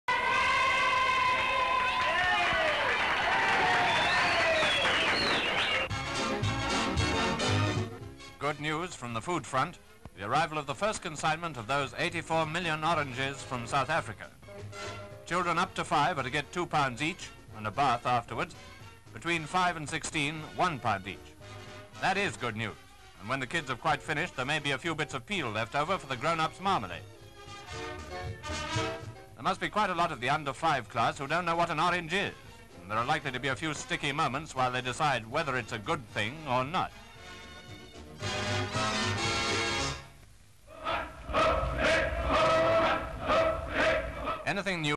84 MILLION -
(18 Oct 1943) The first consignment of 84 million oranges has arrived from South Africa.  Children up to the age of five are to receive two pounds each and from six to sixteen one pound.